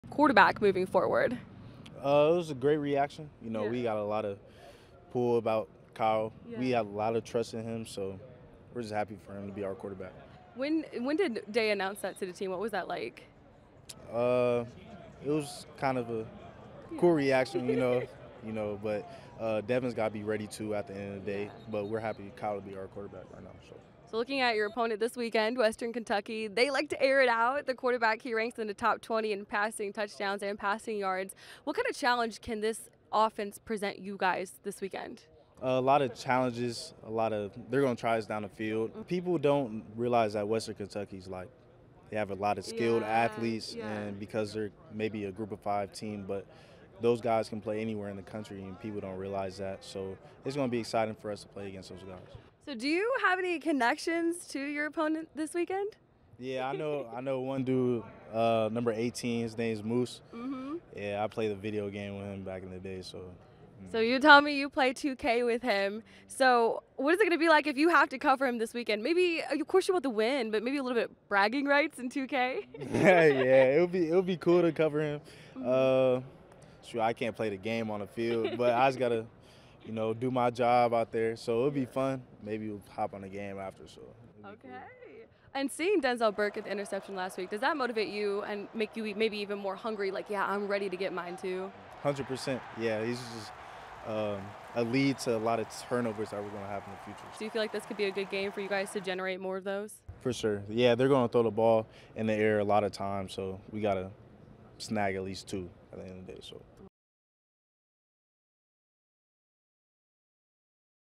One-on-one interview with Ohio State corner Jordan Hancock
Hancock previews Saturday's match up with Western Kentucky